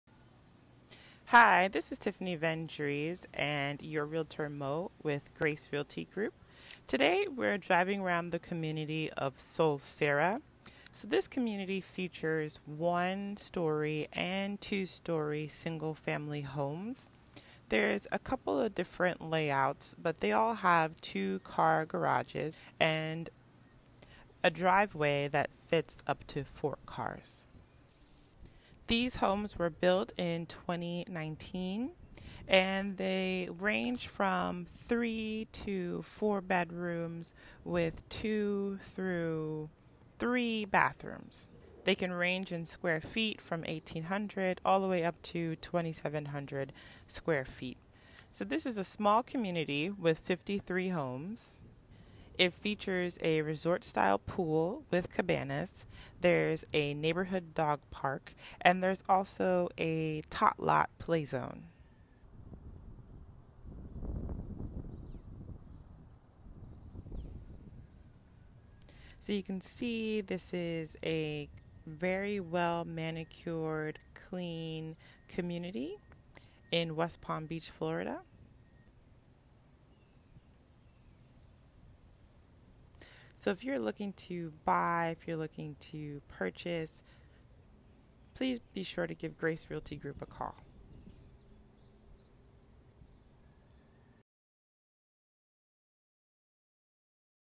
Solcera Community in West Palm Beach, Fl
The Solcera community features one and two story single family homes with two car garages.